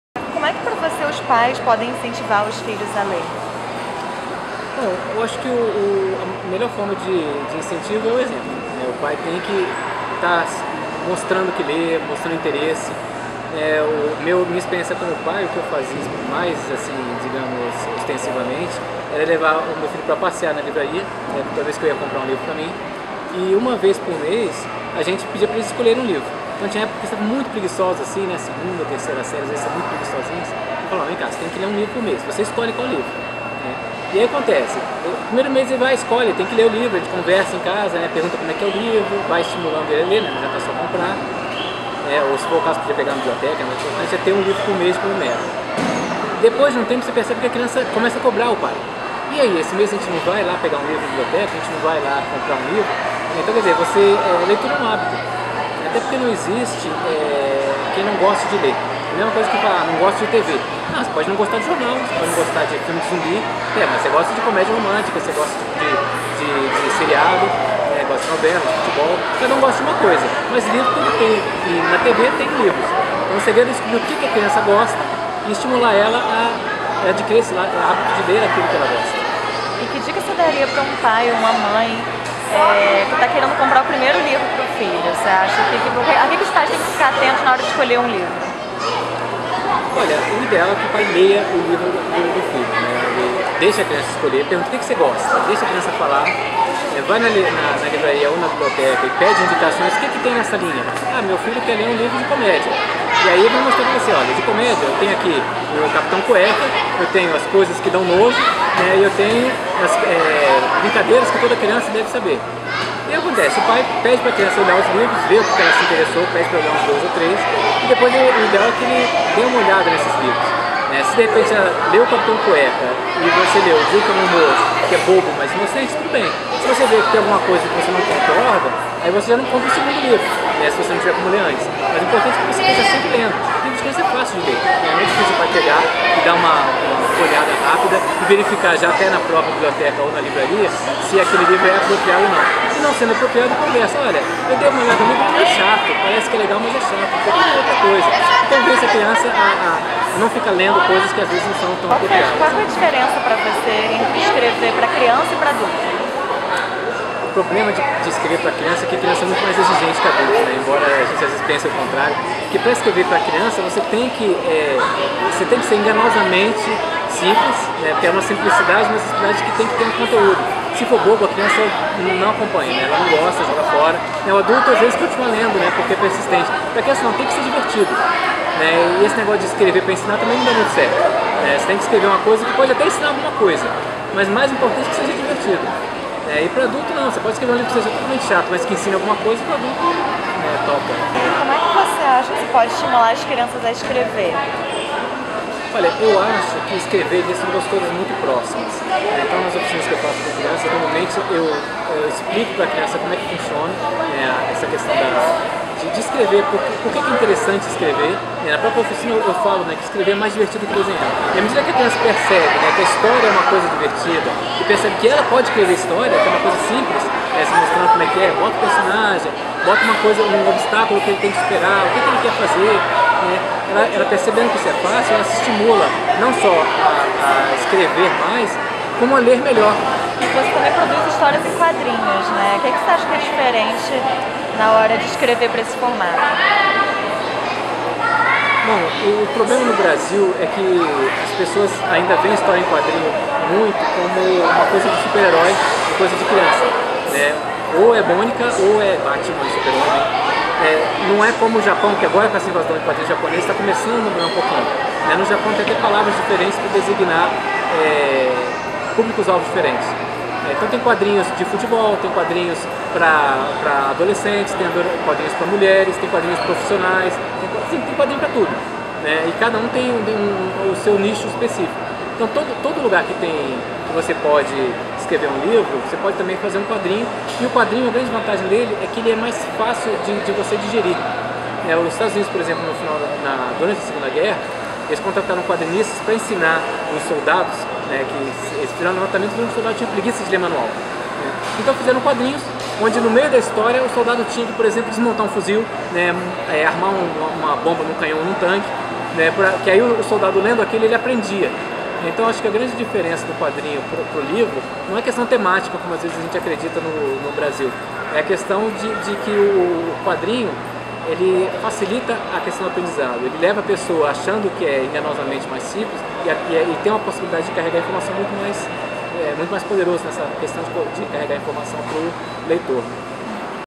Escritor Alexandre Lobão fala sobre literatura para crianças
No bate-papo, o autor falou sobre como estimular os pequenos a ler e escrever, além da potencialidade das histórias em quadrinhos para incentivar esse hábito.